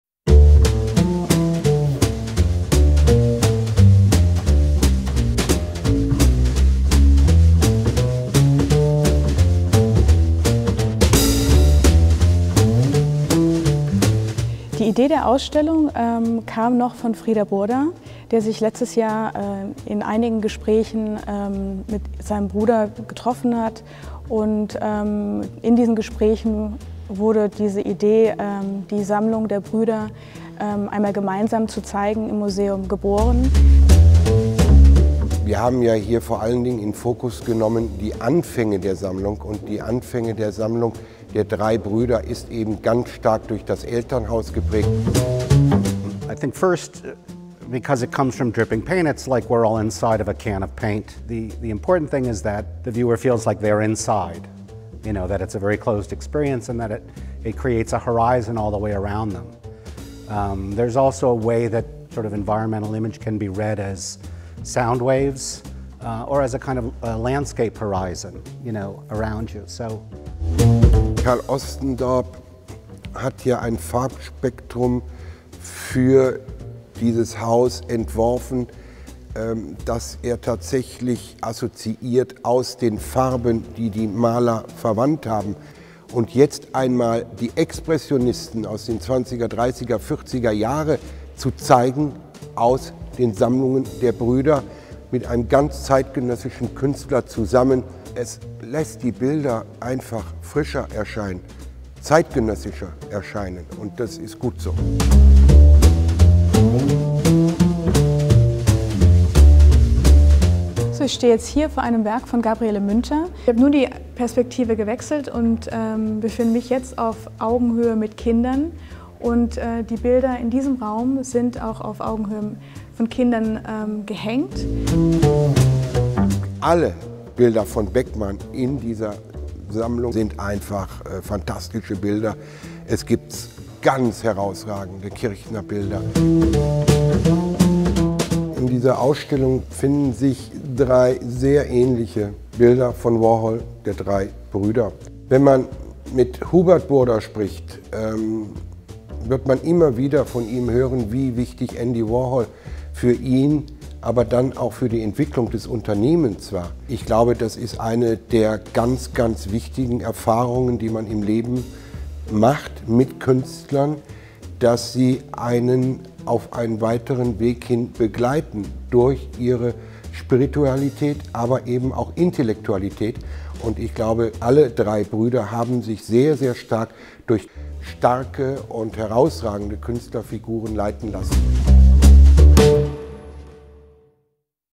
Film zur Ausstellung DIE BILDER DER BRÜDER. Eine Sammlungsgeschichte der Familie Burda
DIE BILDER DER BRÜDER. EINE SAMMLUNGSGESCHICHTE DER FAMILIE BURDA
12. MAI — 4. OKTOBER 2020

Die Familie Burda und die Kunst – eine leidenschaftliche Beziehung. Aber wie fing eigentlich alles an? Was die Eltern Aenne und Franz Burda sammelten, förderte früh die Begeisterung der drei Brüder Franz, Frieder und Hubert für die Kunst. Eine Leidenschaft fürs Leben entstand.

Von Max Beckmann über Ernst Ludwig Kirchner und Karl Schmidt-Rottluff bis zu Gabriele Münter: Es ist der deutsche Expressionismus, dem die drei Brüder Franz, Frieder und Hubert ihre erste Begegnung mit Kunst verdanken. Gesichter leuchten in starkem Pink, Körper räkeln sich in grellem Gelb, schwarze Ränder fassen die Flächen holzschnittartig ein. Dem deutschen Expressionismus verdanken die Farben ebenfalls ihre Emanzipation von den Dingen und der Wirklichkeit, er stellt sie in den Dienst des unmittelbaren subjektiven Ausdrucks von Emotionen, von Seelenwelten und Welterfahrung.

Expressiv und farbig ist auch die Inszenierung der Ausstellung, die die klassisch weiße Architektur des Gebäudes von Richard Meier der Macht der Farben überantwortet. Dazu wurde der amerikanische zeitgenössische Maler Carl Ostendarp (geboren 1961 in Massachusetts) eingeladen. Seine Wandmalerei baut auf ein ausgeklügeltes Farbkodierungssystem. Gleichzeitig zitiert er einen flächigen Comic-Stil, der die Farbe perfekt zur Geltung kommen lässt, indem er sie wie eine delikate Glasur über die Wände laufen lässt. Die davor angebrachten Werke erscheinen als zentrale Etappen im Verlauf dieser imaginären Lebenslinien mit all ihren Höhen und Tiefen. Sie fügen sich ein in die spielerisch entstehenden Kurvungen und Amplituden und werden so humorvoll in ihrer Wirkung bereichert und gesteigert.

Den Auftakt der Ausstellung bildet das bekannte Gruppenporträt The Three Gentlemen der drei Brüder Burda von Andy Warhol, der amerikanischen Pop Art-Legende, in drei farblich unterschiedlichen Varianten. Das gesamte Museum verwandelt sich in einen für den Betrachter erlebbaren, umfassenden Farbkosmos. Im Obergeschoss ist ein eigener Raum den jungen Besuchern gewidmet: Die Kunstwerke hängen auf deren Augenhöhe, wie es auch Andy Warhol für eine seiner Ausstellungen realisiert hat.

Die Ausstellung im Museum Frieder Burda spürt den Wurzeln der Sammlungstätigkeit der drei Brüder nach. Noch zu Lebzeiten von Frieder Burda geplant, spiegelt sie seinen großen persönlichen Wunsch wider, die Kunst der drei Geschwister einmal in seinem Museum zu vereinen.

THE BURDA BROTHERS. A HISTORY OF COLLECTING
2 MAY – 4 OCTOBER 2020

The Burda family and art – a passionate relationship. But where did it all start? The artworks collected by the parents, Aenne and Franz Burda, provided early inspiration to the three brothers Franz, Frieder and Hubert, who would all develop a lifelong love of art.

From Max Beckmann through Ernst Ludwig Kirchner and Karl Schmidt-Rottluff to Gabriele Münter: it is to German expressionism that we owe the three brothers' first introduction to art. Faces glow in bright pink, bodies writhe in glaring yellow, black borders outline the works like wood carvings. It is also German expressionism to which the colors owe their emancipation from things and reality, which put them into the service of direct, subjective expressions of emotions, of mental states and life experiences.

Expressive and colorful is also how one could describe the exhibition, in which the classic white architecture of the museum, built to plans by Richard Meier, defers to the power of colors. To achieve this, the American contemporary painter Carl Ostendarp (born 1961 in Massachusetts) was invited to design the setting. His wall paintings are based on a sophisticated color coding system. At the same time, he cites a two-dimensional comic style, which does justice to the colors by making them look like a coat of delicious icing. The works hung on the walls seem like central stages along these imaginary lifelines, with all their ups and downs. They fit perfectly into the playfully created curves and amplitudes, humorously enriching and amplifying their effects.

The exhibition begins with the well-known group portrait of the three brothers Burda by the American pop art idol Andy Warhol, The Three Gentlemen, in his three different-colored versions. The entire museum is transformed into a comprehensive cosmos of color for the visitor to experience. There is also an extra room upstairs for younger visitors, where the artworks will be hung at their eye level, just like Andy Warhol once did with one of his own exhibitions.

The exhibition in Museum Frieder Burda traces the roots of the three brothers' collecting activities. It was planned while Frieder Burda was still alive and reflects his great personal desire to unite the three siblings' art in a joint exhibition at his museum.